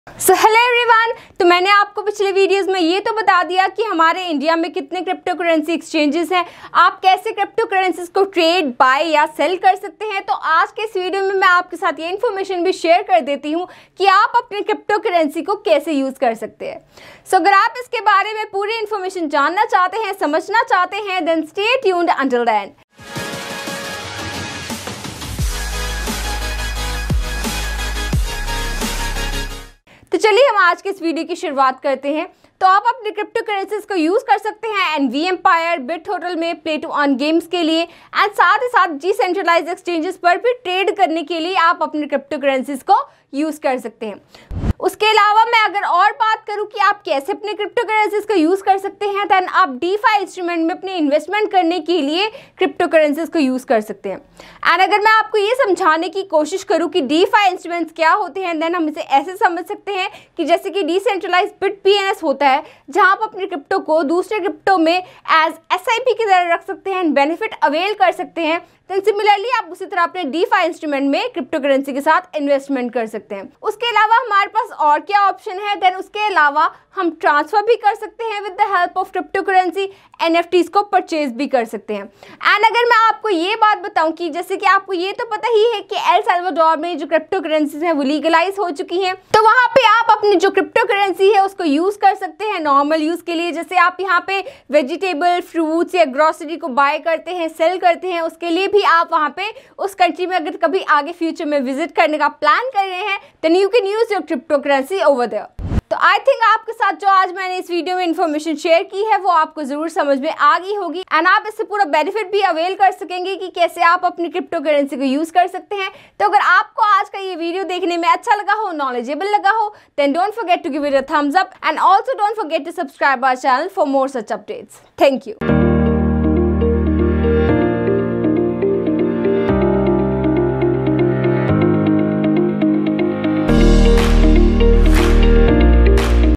Looking for ways to use your cryptocurrency? | How to use your cryptocurrency explained
Are you also looking for ways to use cryptocurrency?

Where and how you can use your cryptocurrency?

Then watch this video till the end to know how you can use your cryptocurrency

 Explore from a variety of crypto education courses at  👇🏻

~Don't miss any crypto update and receive every one of them in real-time on our Telegram Channel 👇🏻